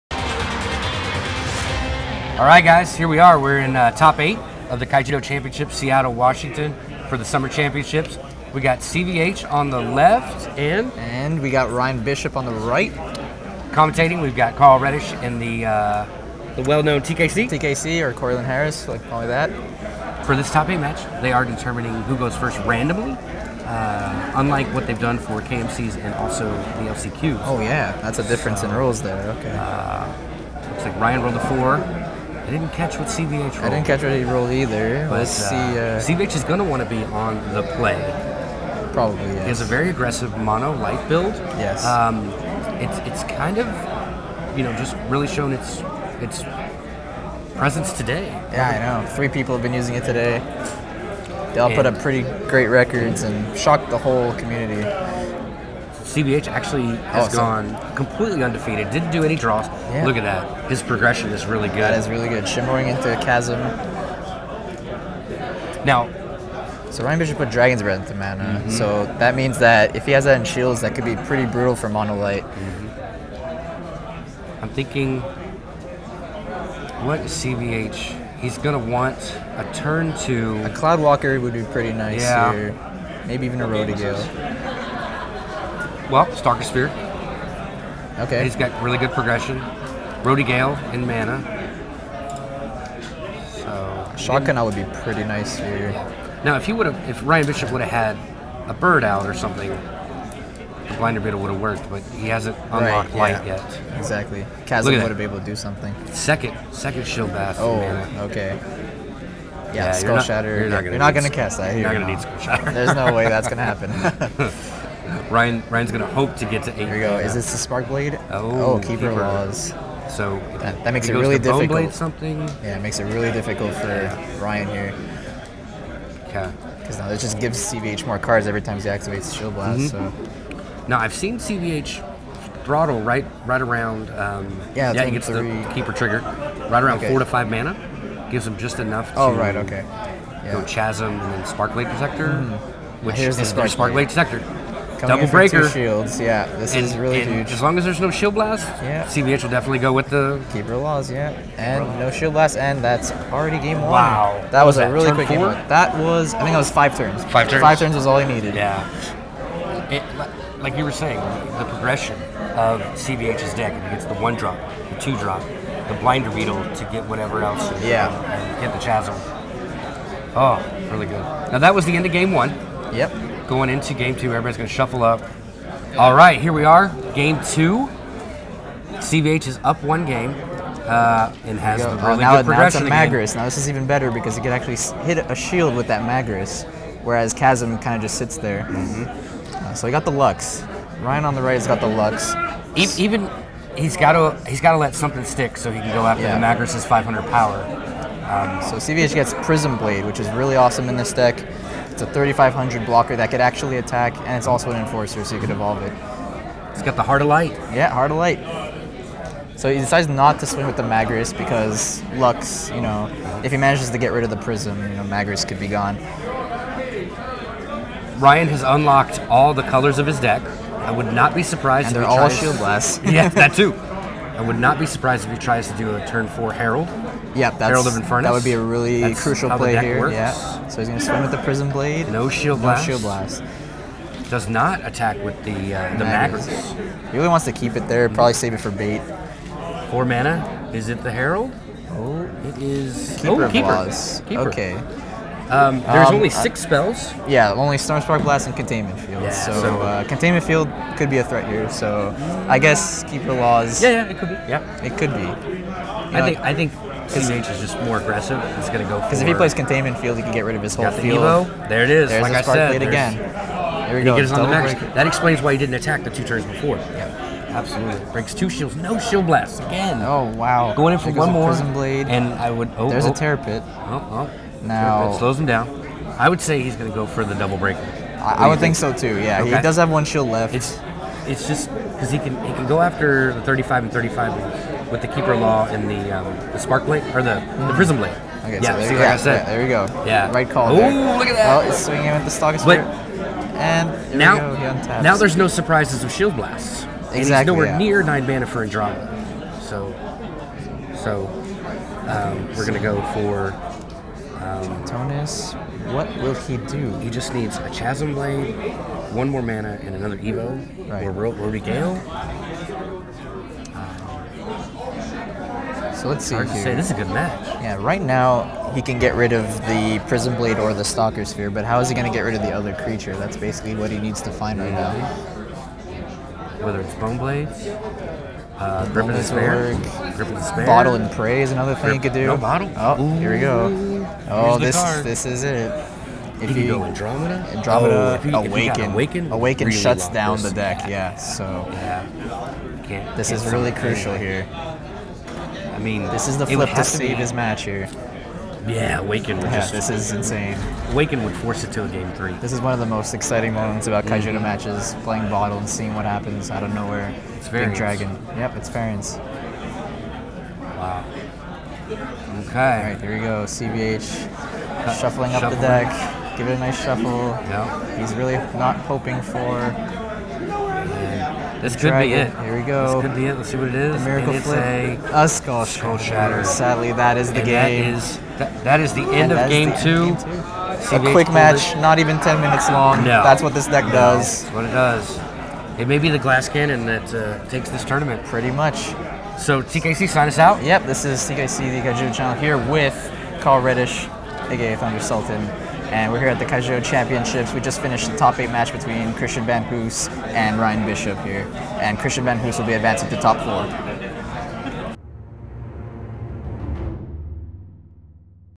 Feature Match - 2013 Summer Championship - CVH vs. Ryan B.
See how these decks did against one another at the Kaijudo 2013 Summer Championship in Seattle! (kaichamps)

CVH (Mono Light) Deck List
3 Chasm Entangler
3 Magris the Magnetizer
3 Shimmerwing
3 Cloudwalker Drone
3 Stalker Sphere
3 Prism-Blade Enforcer
3 Rodi Gale, Night Guardian
3 Blinder Beetle
3 Shaw K'Naw
3 Sparkblade Protector
2 Halon, Paragon of Light
3 Containment Field
2 Stormspark Blast
3 Keeper of Laws

Ryan B. (5-Civ Control) Deck List
3 Logos Scan
3 Skull Shatter
3 Keeper of Laws
3 Nix
3 Crystal Memory
3 King Tritonus
3 Root Trap
2 Hyperspeed Dragon
3 Herald of Infernus
2 Dragon's Breath
3 Infernus the Awakened
2 Infernus the Immolator
3 Bone Blades
3 Andromeda of the Citadel
3 Lyra, the Blazing Sun
3 Lux
3 Terror Pit
3 Bottle of Wishes
3 Stormspark Blast
3 Piercing Judgment

Congratulations to both Ryan and Christian for a great match, and for making it to the Top 8 at the first Kaijudo Championship event!